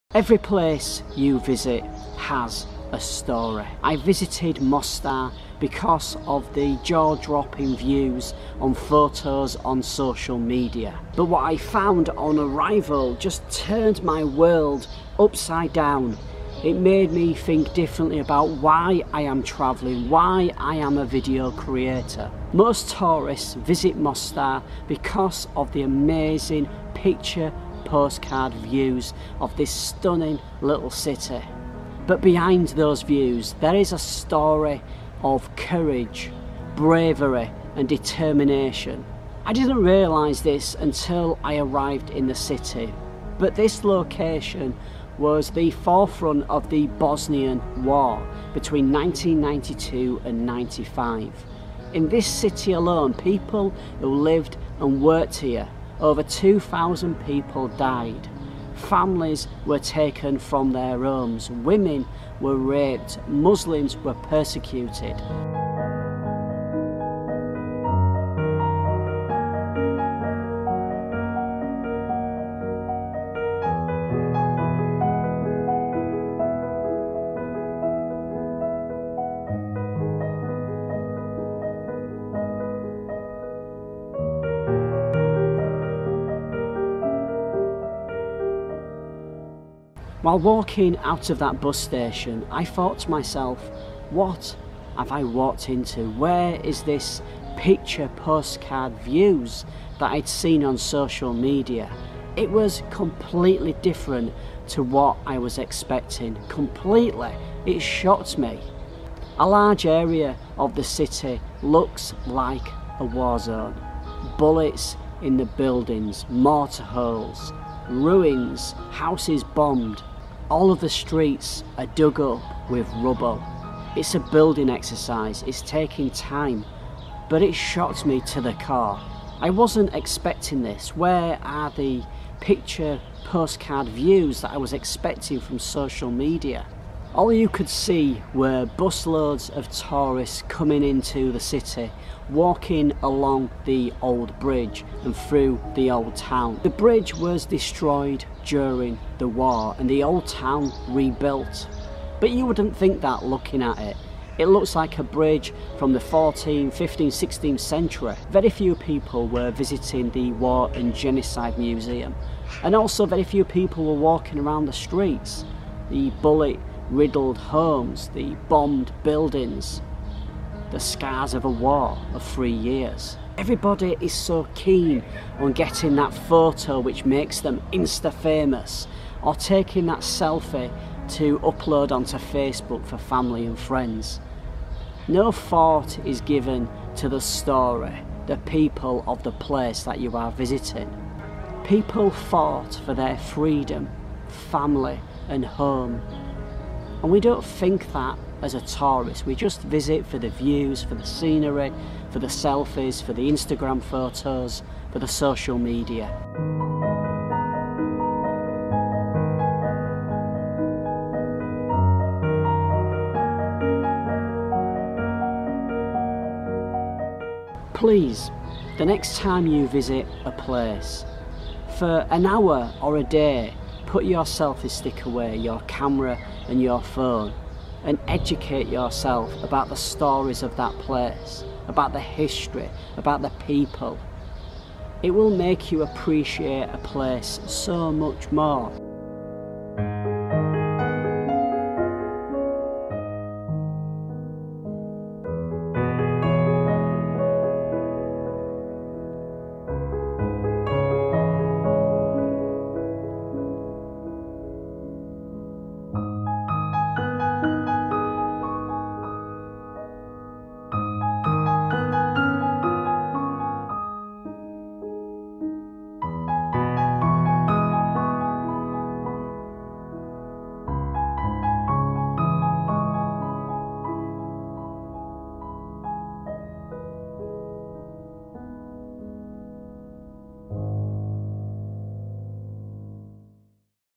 The Story of Mostar, Bosnia & Herzegovina
The Story of Mostar, Bosnia & Herzegovina.  Hope you enjoy this new series that I am working on. Not sure what to call it yet, but it's episode 1, so will obviously improve as we go forward. Let me know what you think of the topic and of course, the storytelling.